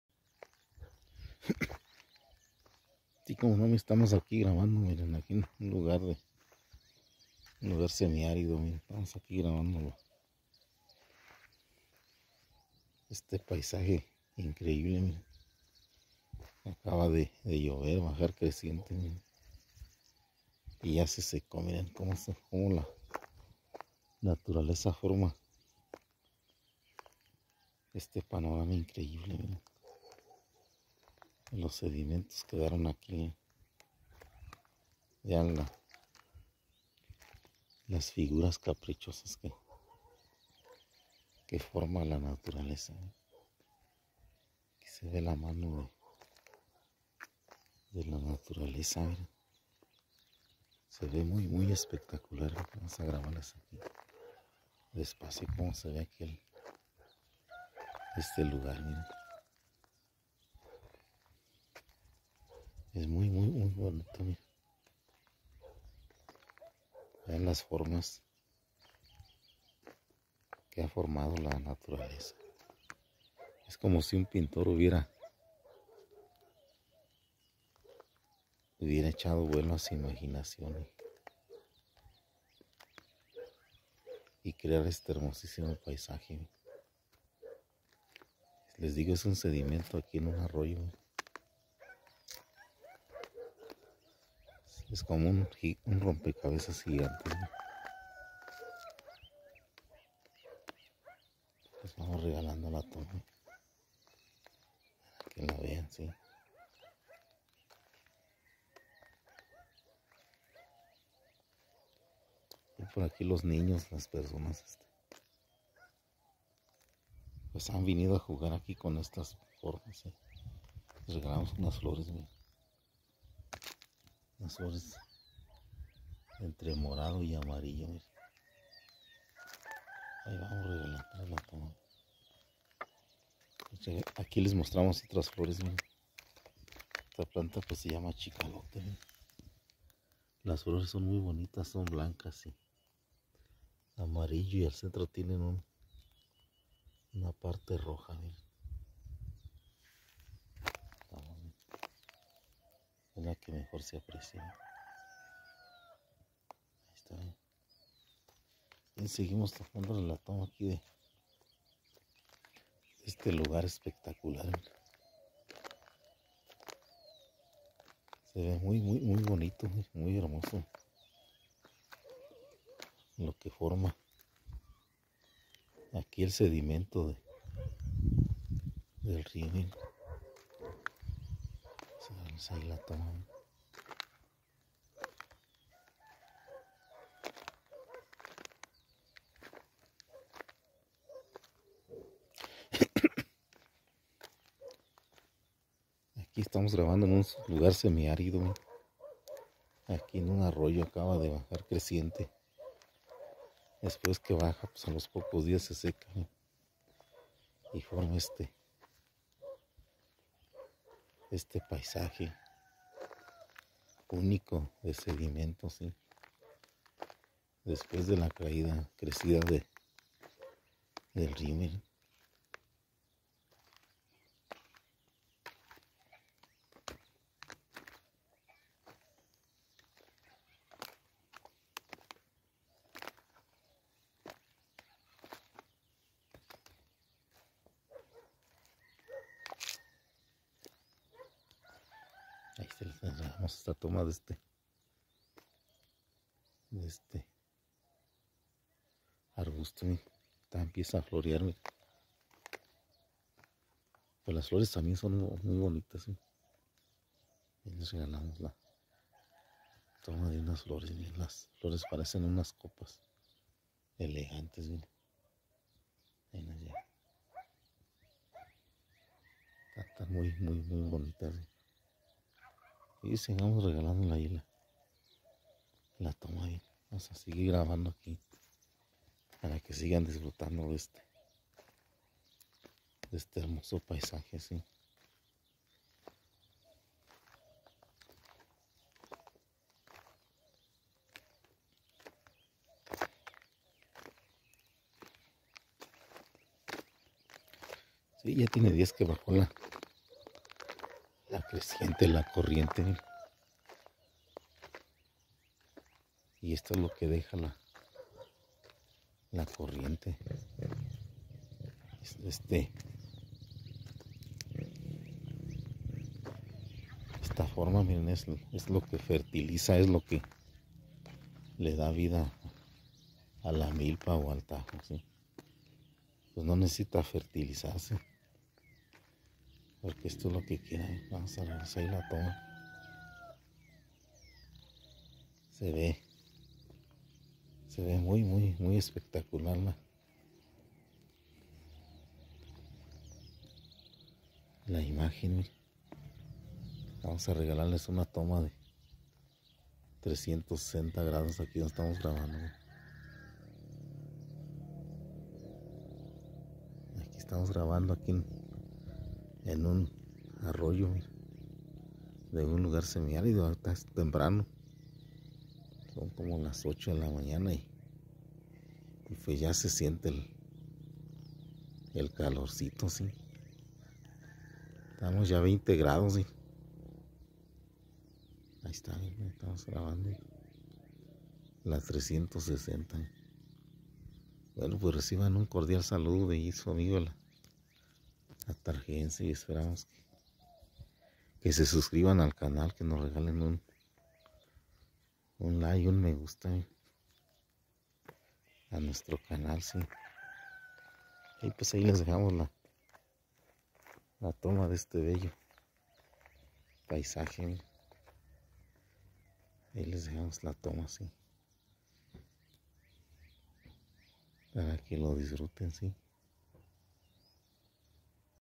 Abono para la milpa, sedimeinto de creciente
El sedimento en Atarjea, se usa omo un abono para las milpas que estan al margen del Río Atarjea, se utiliza un canal o sequia para hacer ingresar el sedimento a la milpa en época de crecientes o cresidas del río.
Los sedimentos son arena, arcilla, limo y otras partículas sueltas del suelo que se depositan en el fondo de una masa de agua. Pueden provenir de la erosión del suelo o de la descomposición de plantas y animales. El viento, el agua y el hielo pueden transportar estas partículas hasta los ríos, lagos y arroyos.